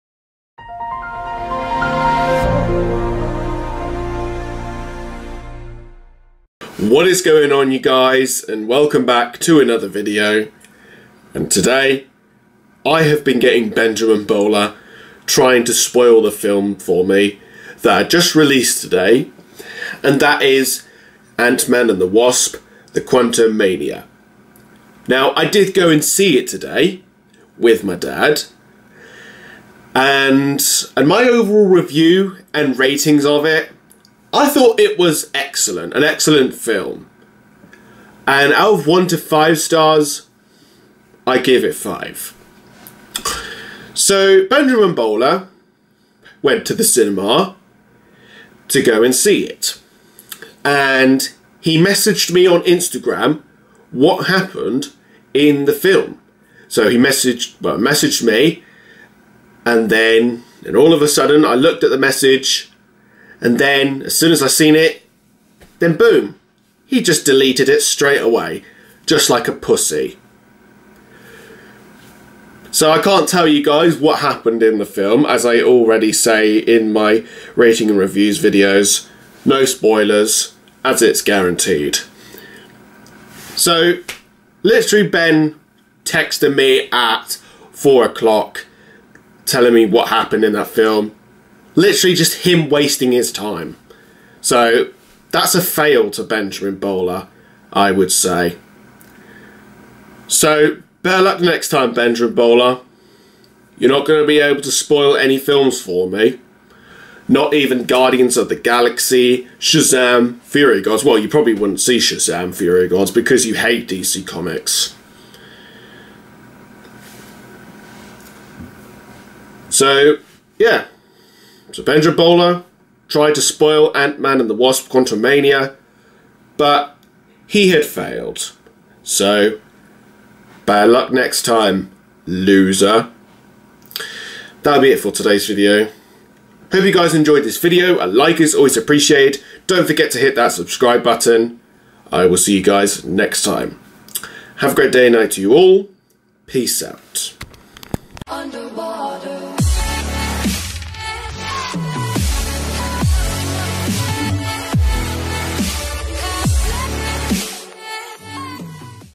Ben Bowler tried to spoil Ant-Man and the Wasp: Quantumania
Ben Bowler fails to spoil the film I've already seen after It was released.